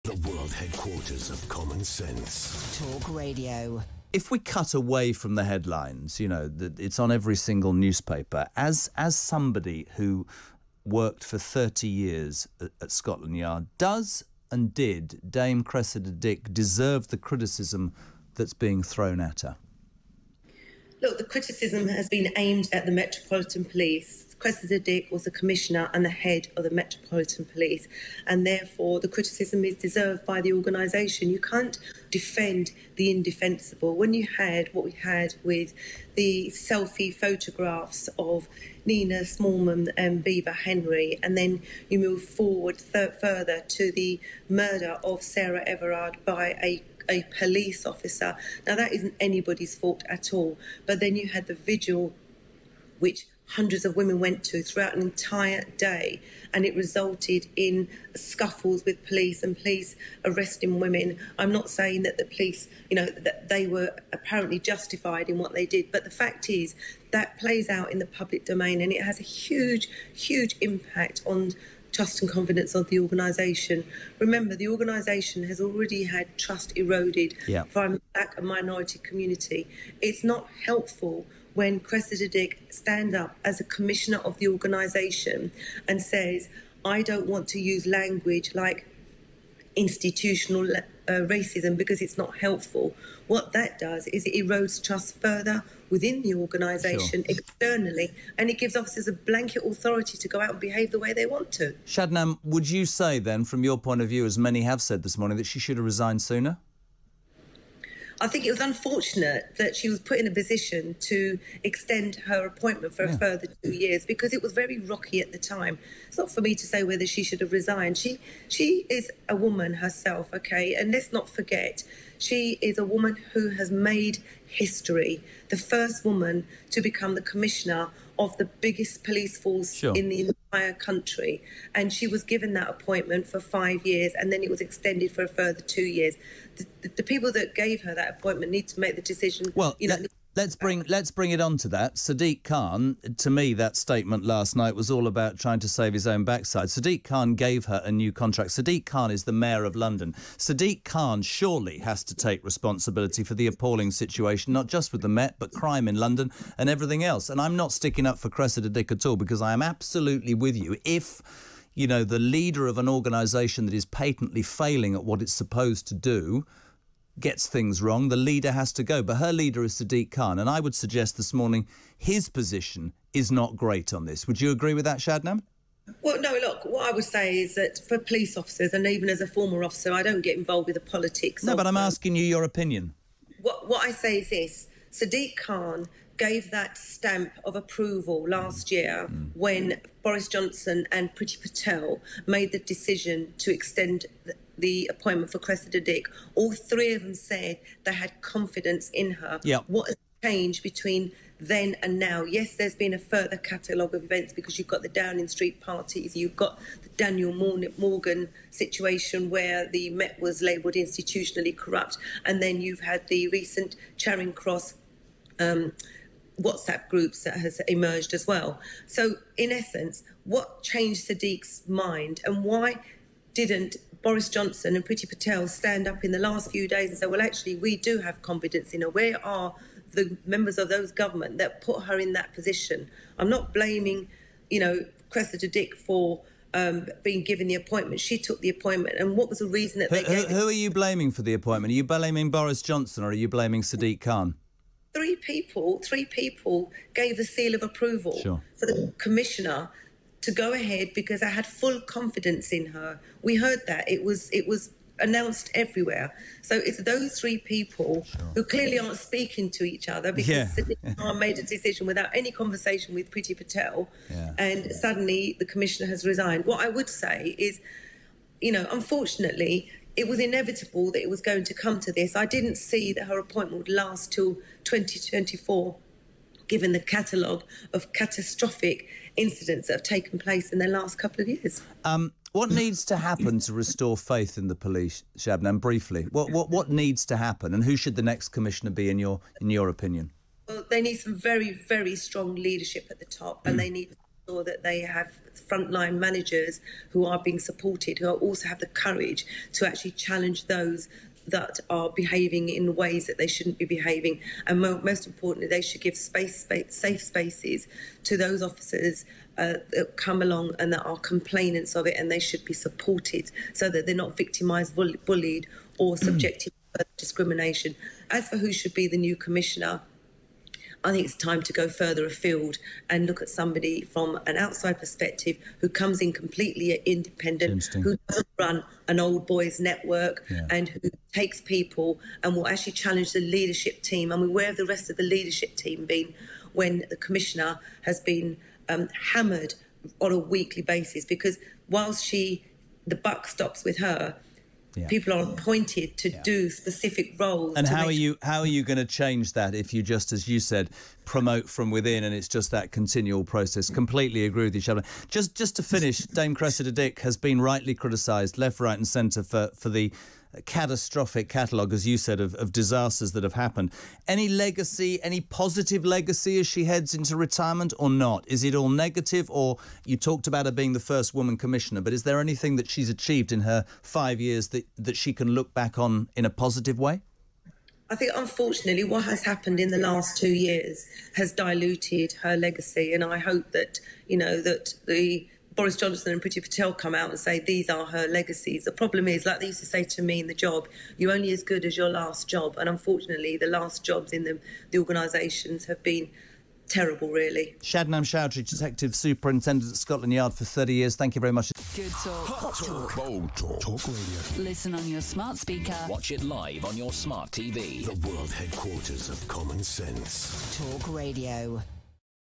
Dame Cressida Dick has been forced out as Met Police Commissioner
Ex Scotland Yard Detective Superintendent Shabnam Chaudhri: "You can't defend the indefensible. It was inevitable given the catalogue of catastrophic incidents that have taken place."